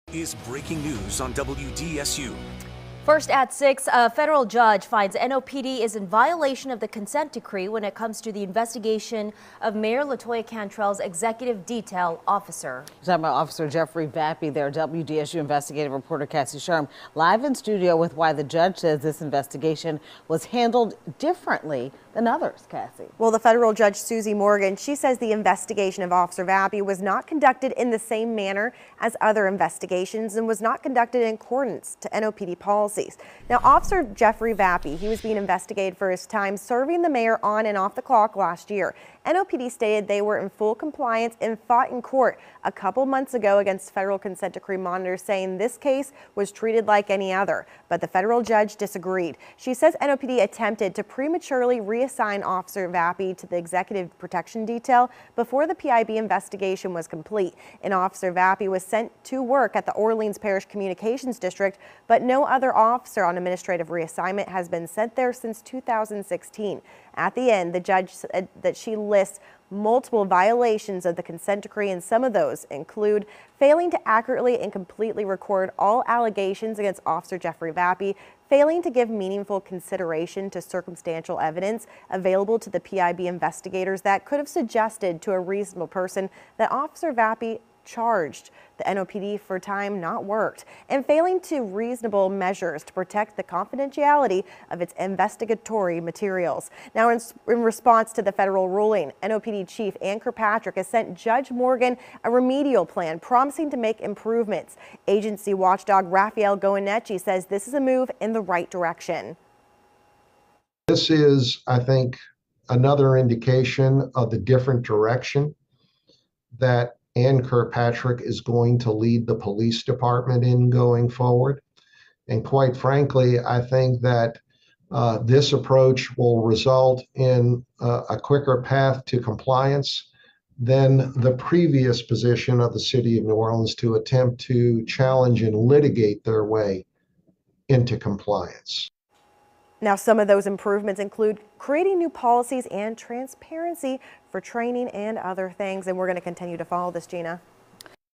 Federal Judge says PIB violated consent decree with Officer Vappie investigation
In response, NOPD Superintendent Anne Kirkpatrick has sent Judge Morgan a remedial plan.